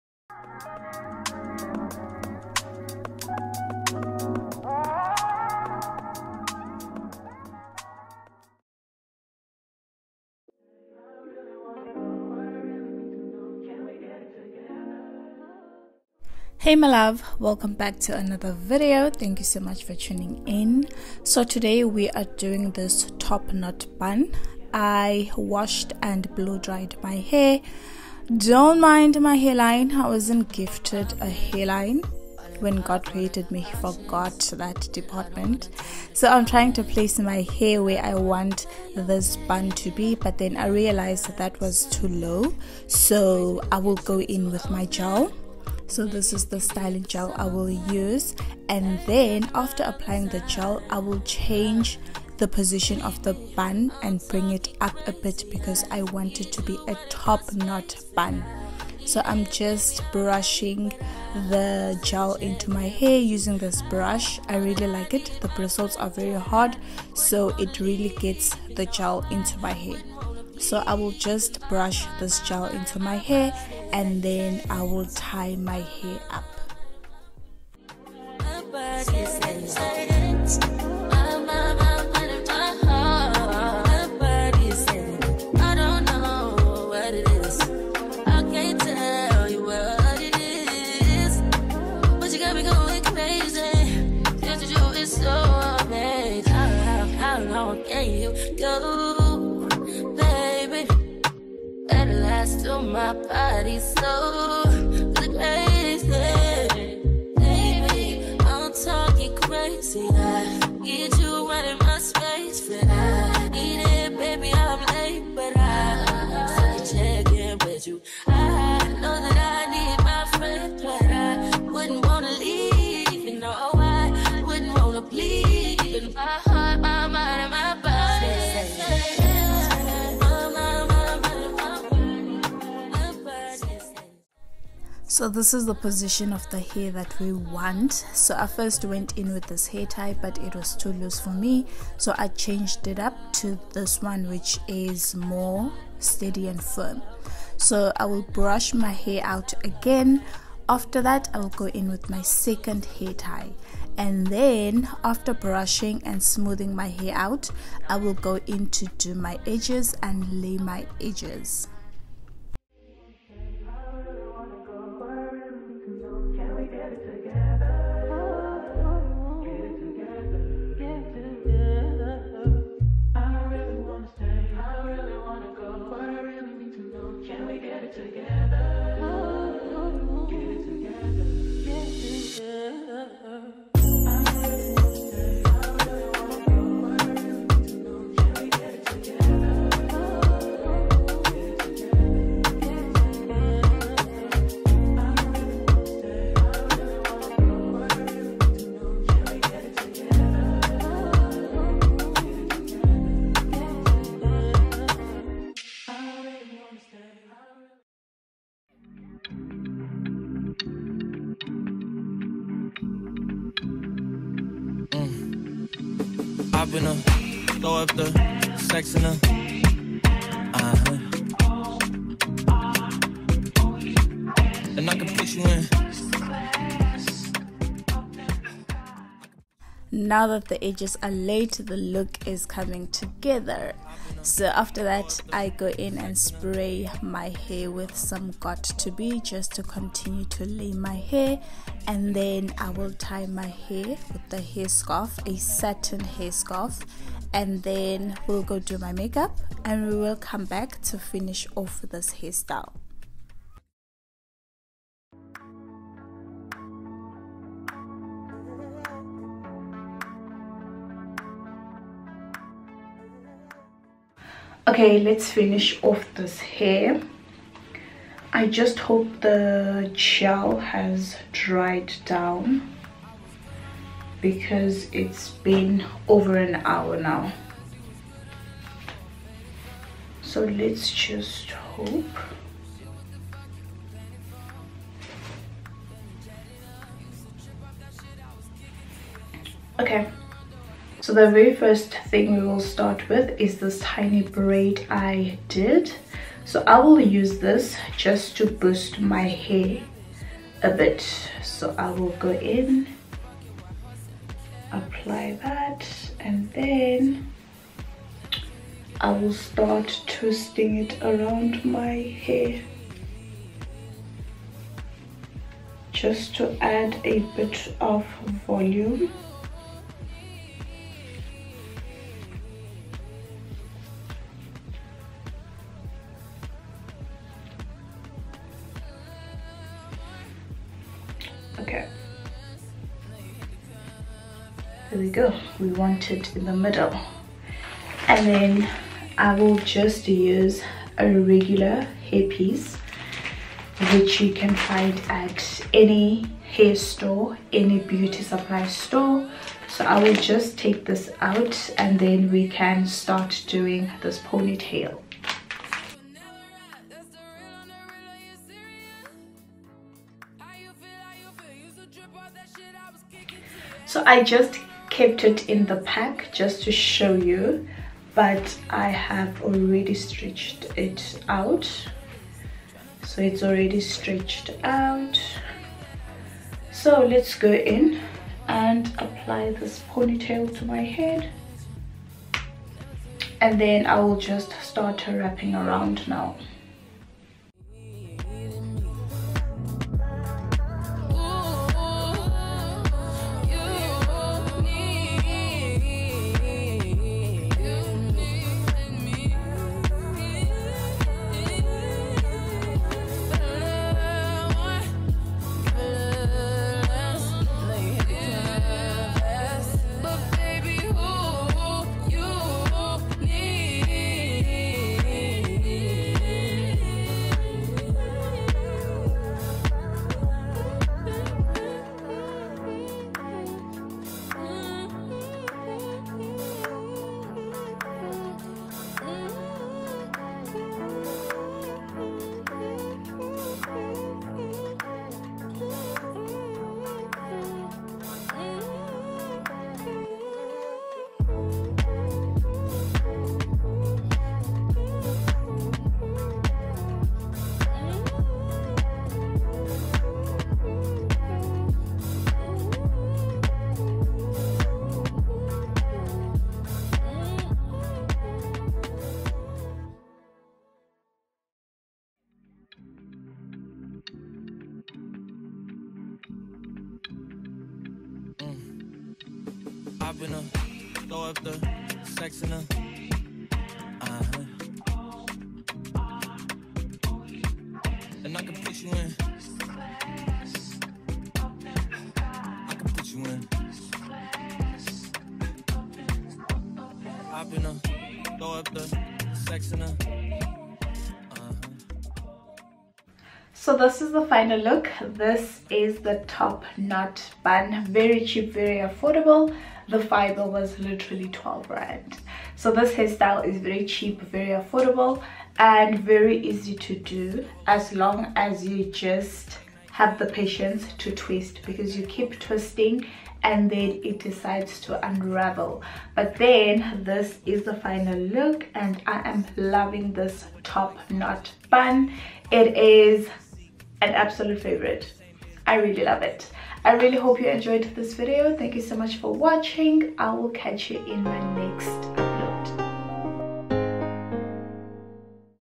Sleek Top Knot Bun Using Braiding Hair | Zuki Williams
Hey lovelies🤍 Welcome or welcome back. In today's video I will be doing a get ready with me. I hope you enjoy lovers 🤍

Shop IG: @laura_luxecollection

South African Vlogger 🖤